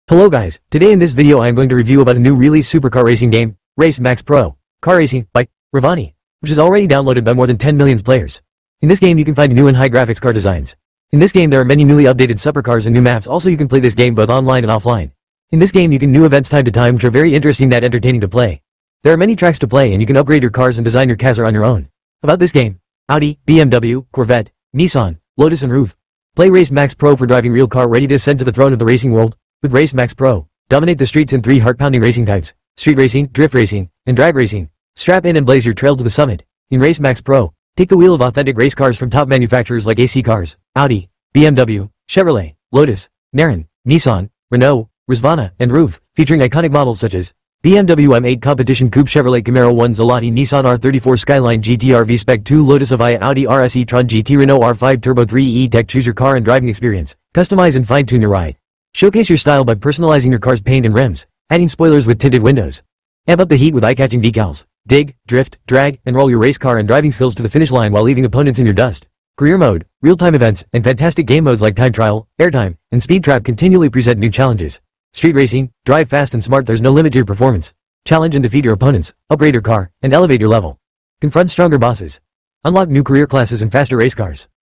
REVIEW - RACE MAX PRO : CAR RACING - NEW HIGH GRAPHICS CAR RACING GAME - WORTH PLAYING?
REVIEW - WORTH PLAYING OR NOT - RACE MAX PRO : CAR RACING - NEW HIGH GRAPHICS CAR RACING GAME

car racing games for po
can racing games offline
car racing games for po low end